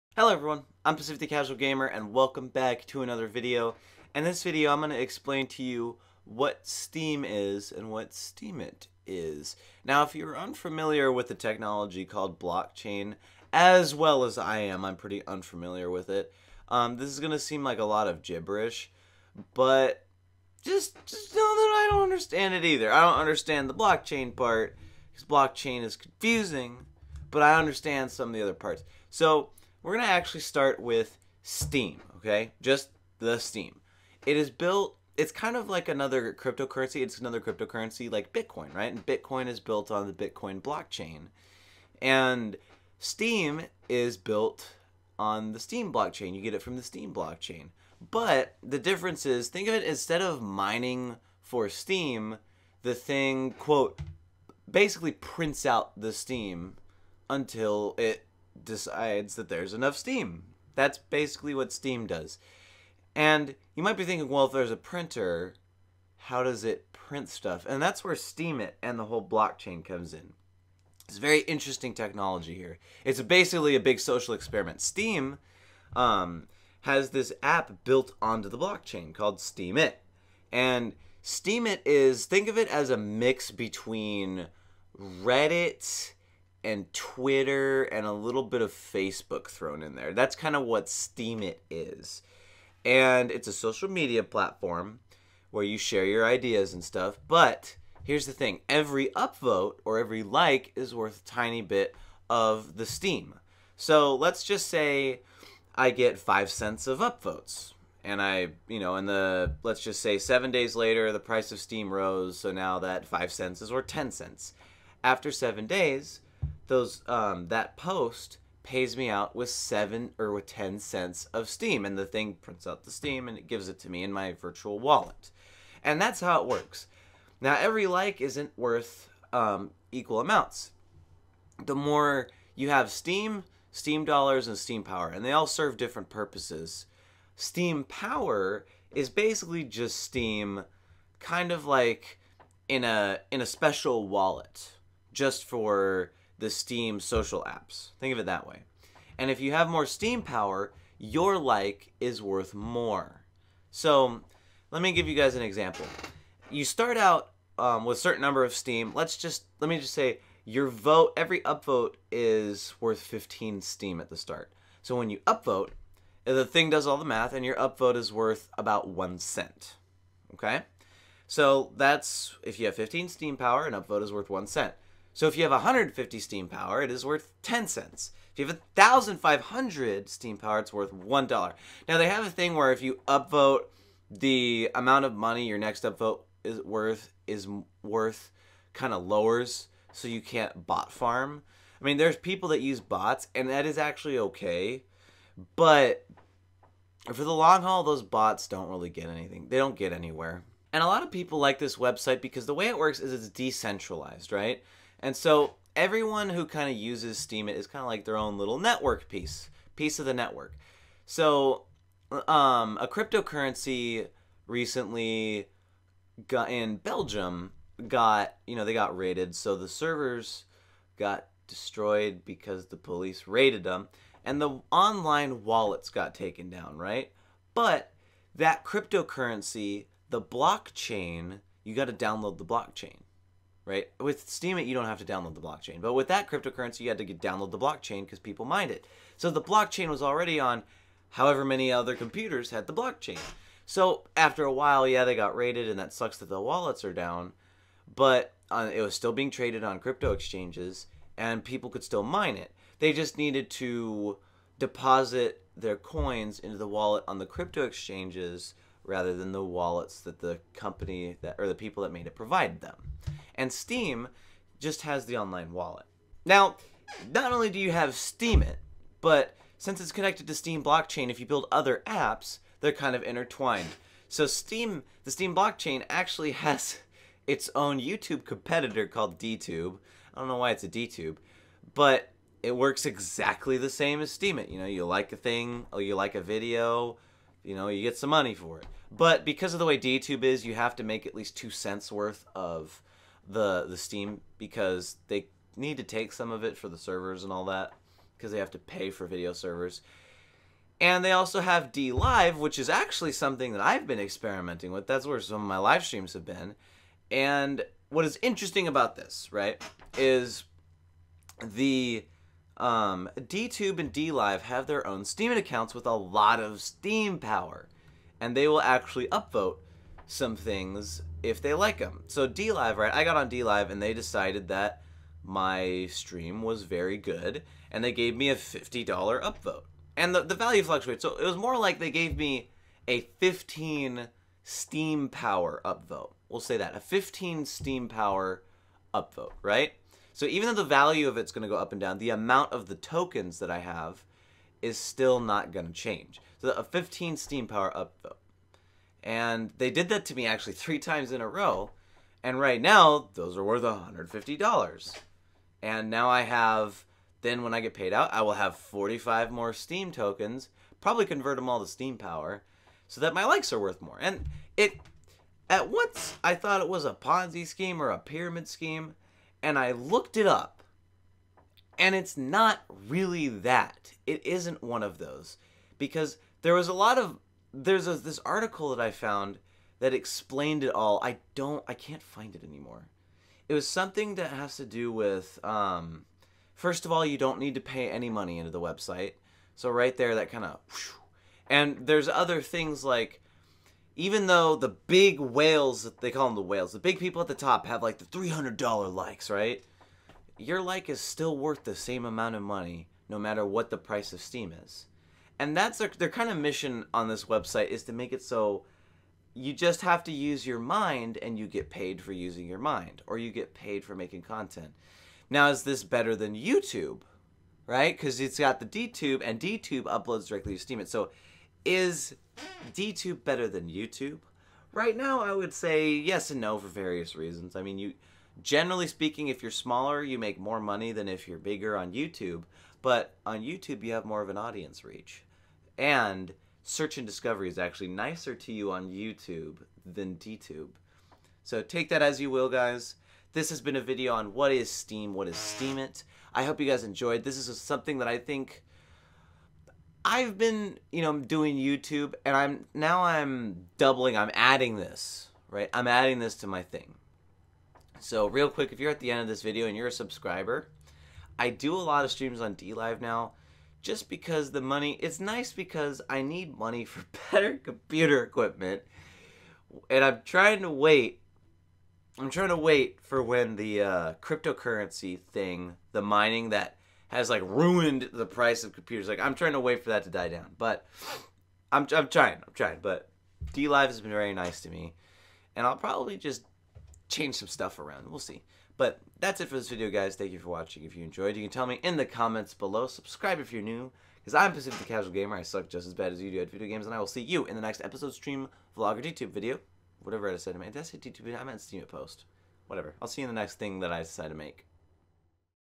What is STEEM and Steemit?
Some of you out there on the internet may have heard of this new thing called Steem, or maybe have heard of a bogging website called Steemit.  I'm here to explain it them both to you.

Steem is a blockchain and a crypto currency.  However, Steem is not mined like Bitcoin or Etherium, it is more closer to printed.  Steem is earned when someone upvotes something, whether it's your own content or a comment.  Since Steem is a crypto currency, the price does fluctuate.

(I post original content there!)